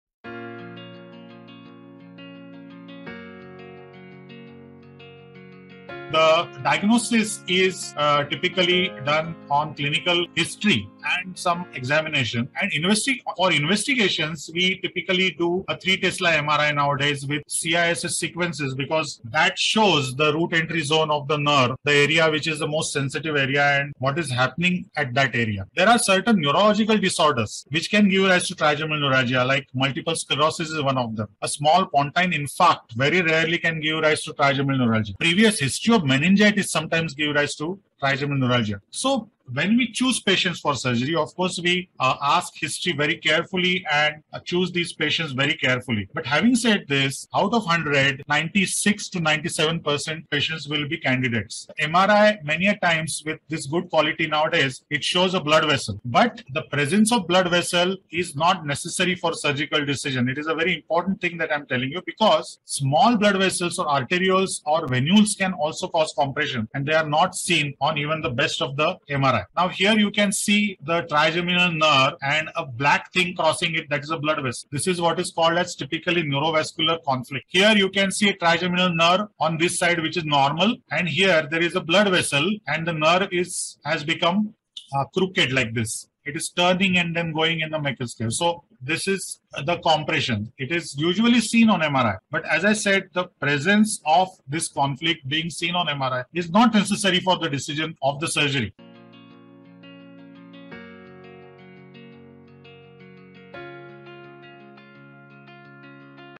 How to Diagnose Trigeminal Neuralgia? Lecture by Dr Jaydev Panchwagh.
How to diagnose trigeminal neuralgia?
Often it is a clinical diagnosis, supported by an MRI of the trigeminal nerve area.